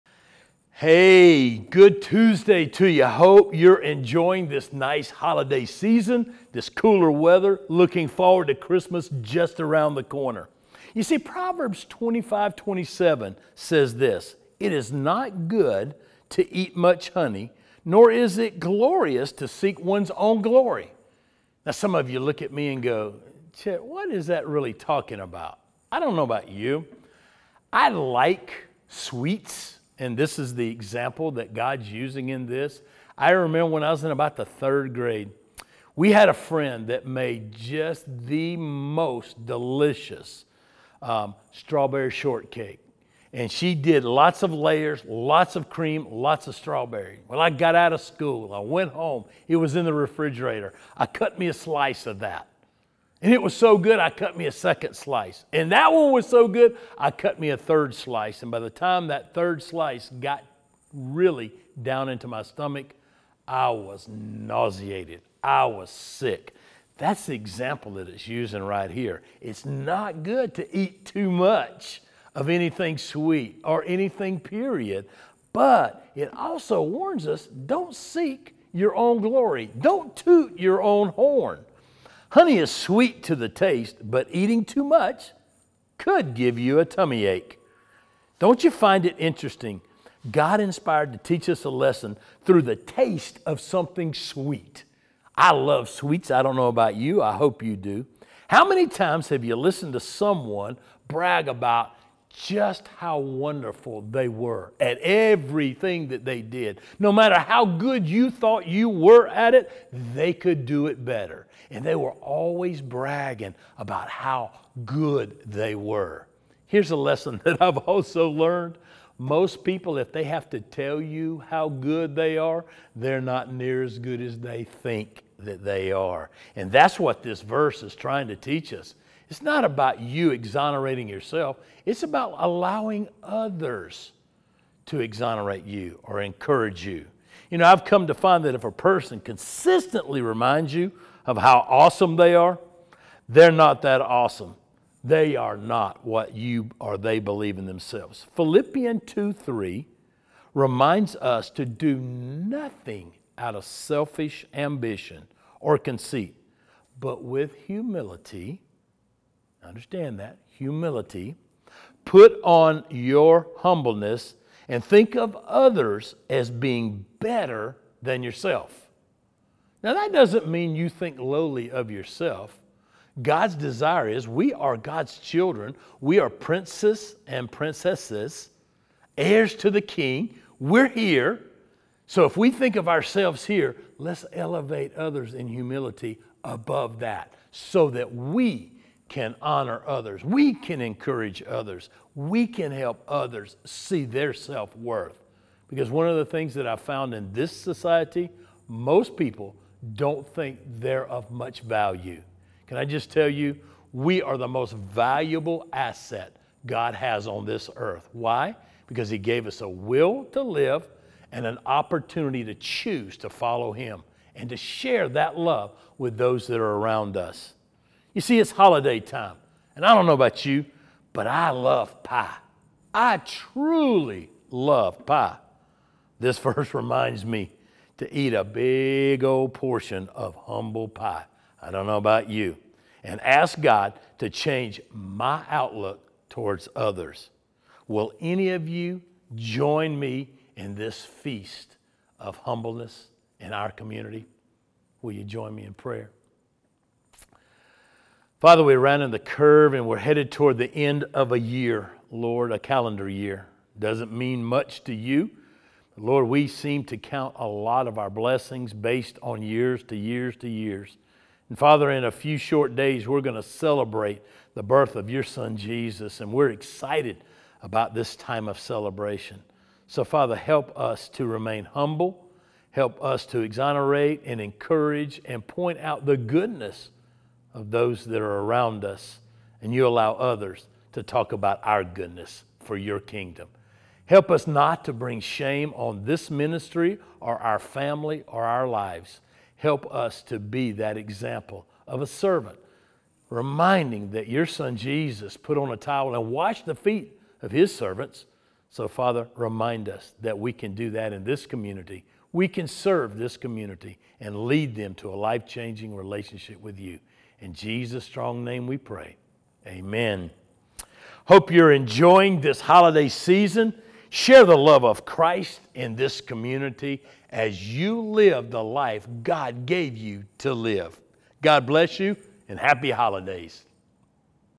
Your Word for the Day - Episode 446
Pastor Chet encourages us to practice humility and to elevate others rather than ourselves.